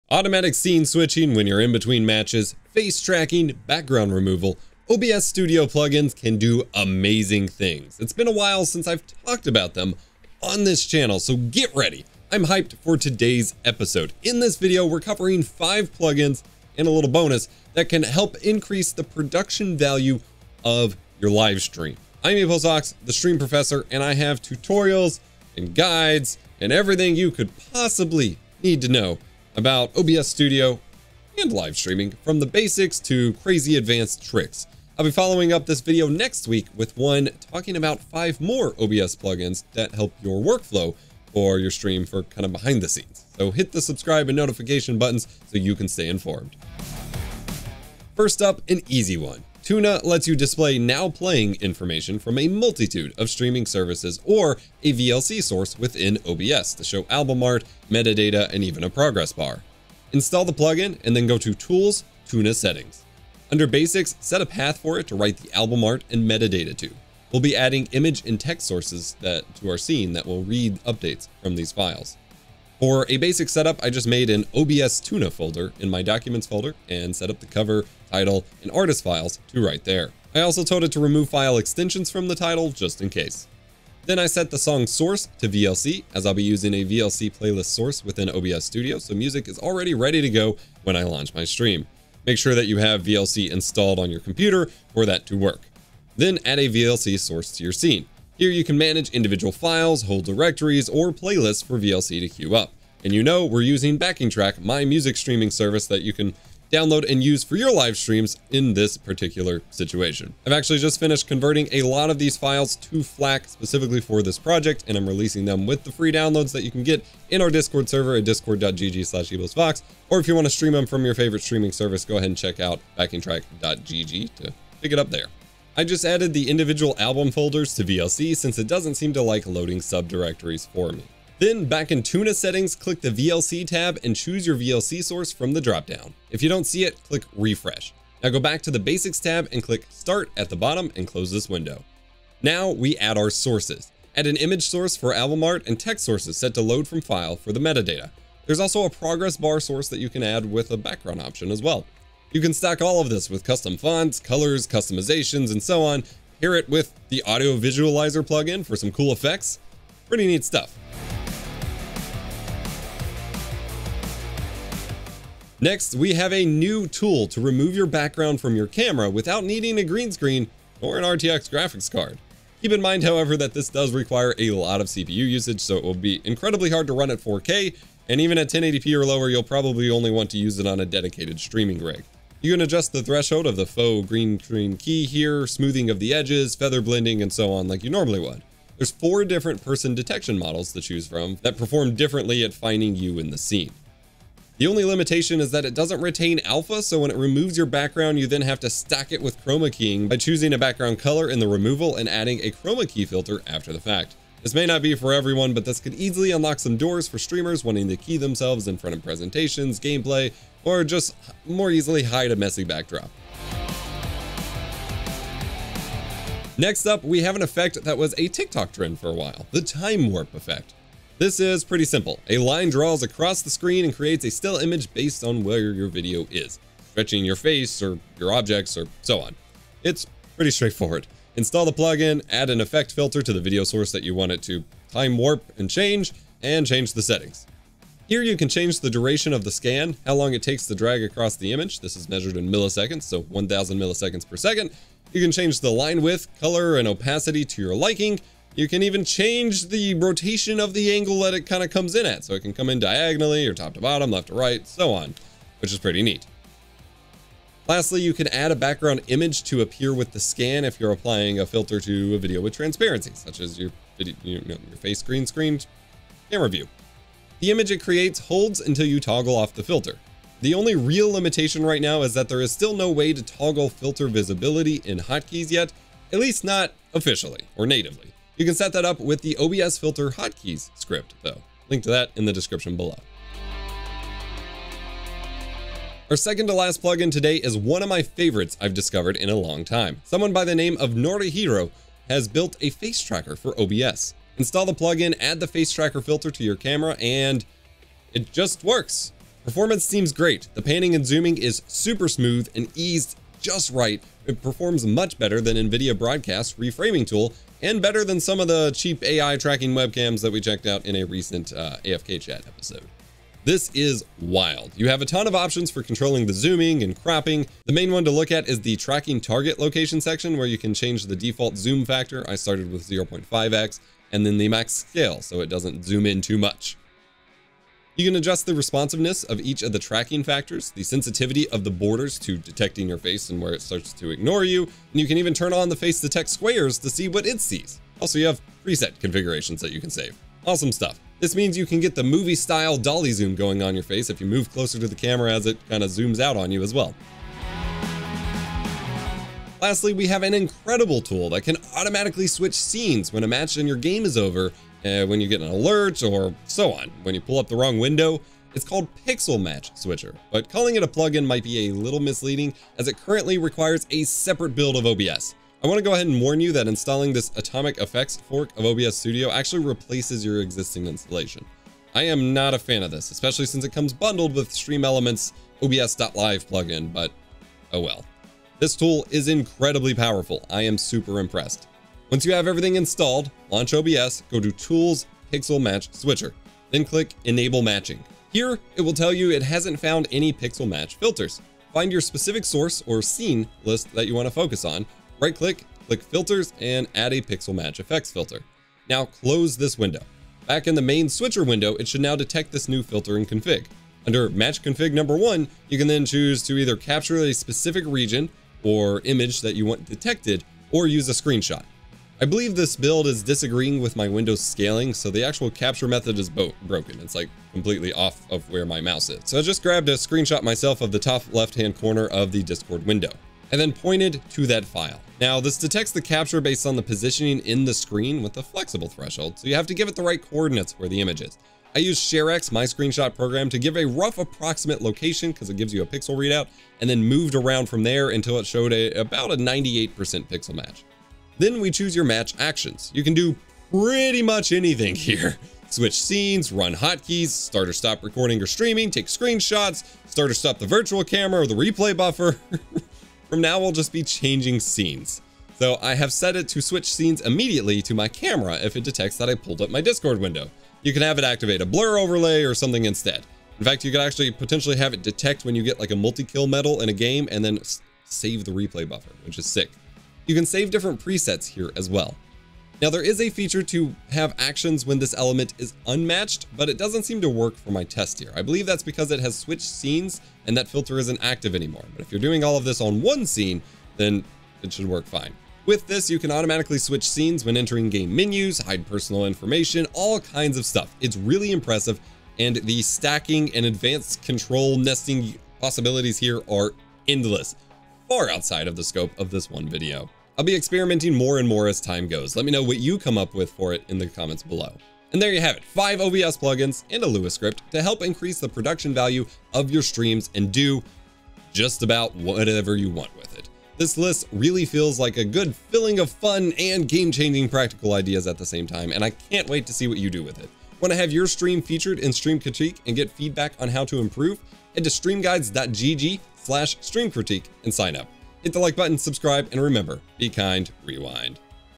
5 OBS Plugins to Make Amazing & Unique Streams!
Today we're looking at 5 awesome OBS Studio plugins that will help improve the production value of your streams!

TIMECODES:
00:00 Intro
00:46 Now Playing
02:43 Background Removal
03:33 Time Warp
04:36 OBS Filter Hotkeys
04:47 Face Tracker
05:48 Pixel Match Switching
08:24 Conclusion

 🔥 Support 🔥
(The best way to support me right now)